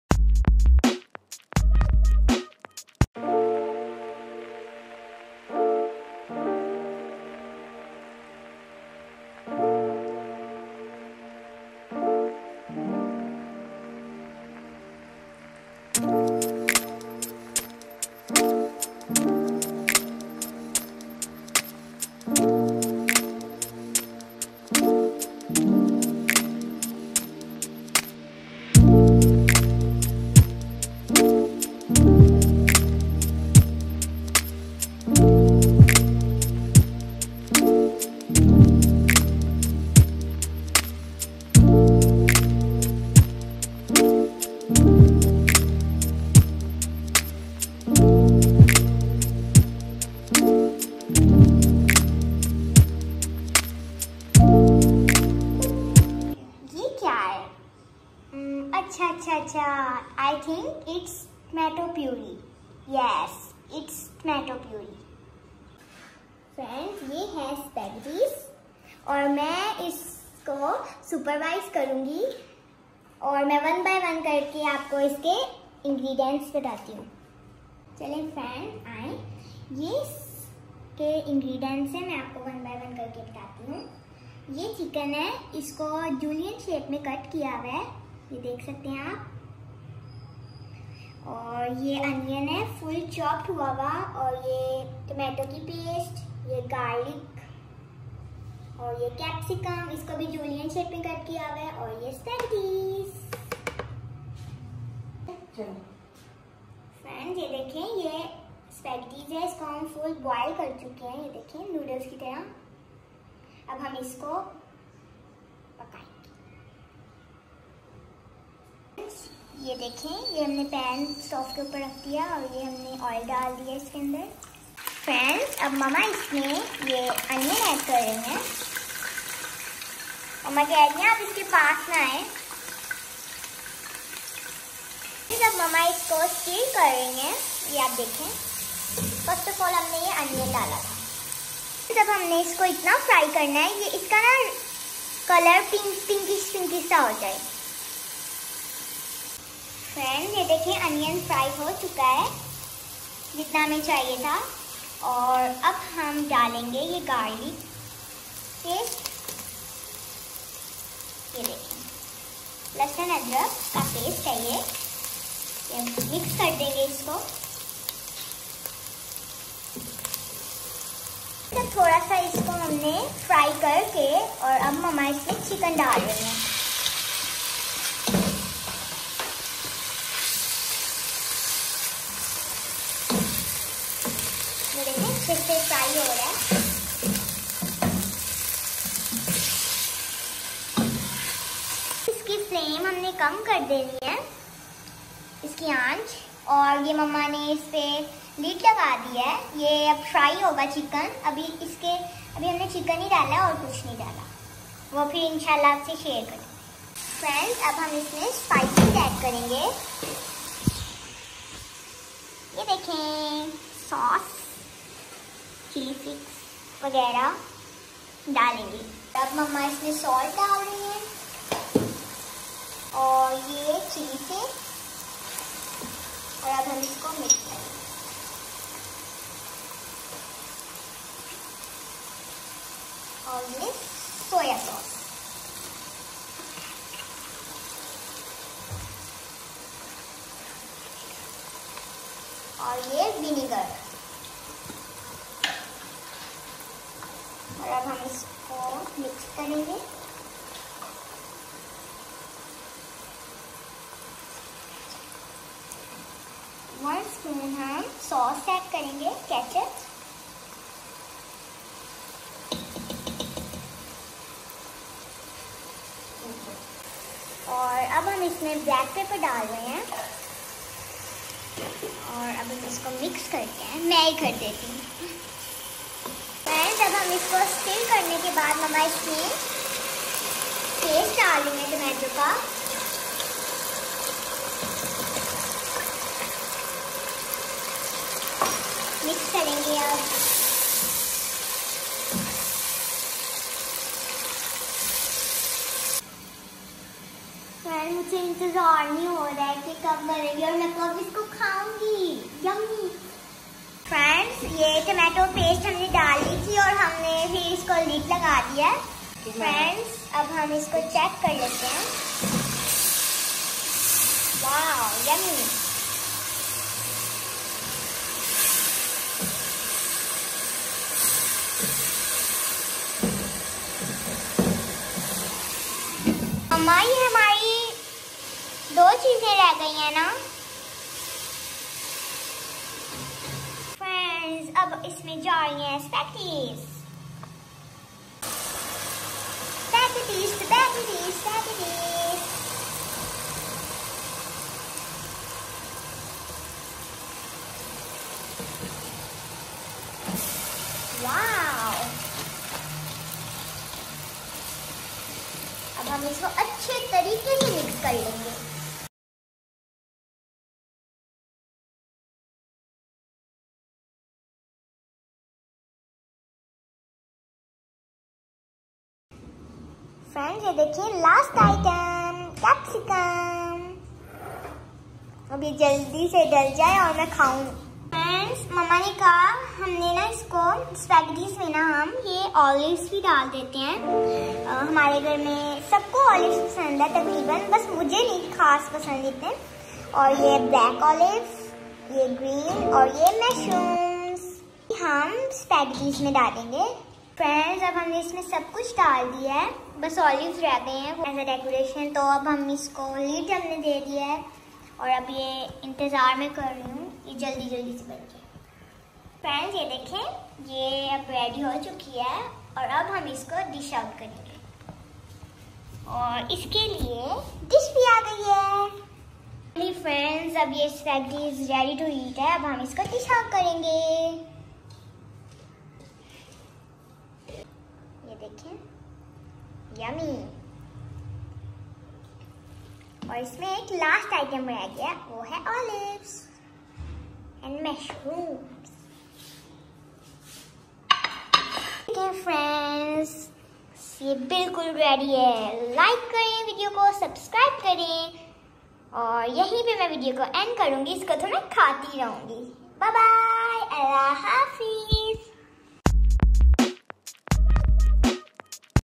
CHICKEN SPEGGATY🍝🍜 by Ayat inspiration ❤️❤️🤲
"Check out this delicious and easy CHICKEN SPEGGATY recipe by Ayat inspiration! Learn how to make a mouthwatering chicken speggaty dish that will delight your taste buds. This step-by-step tutorial will guide you through the process. CHICKENSPEGGATY SpeggatyRecipe AyatInspiration EasyDinnerIdeas"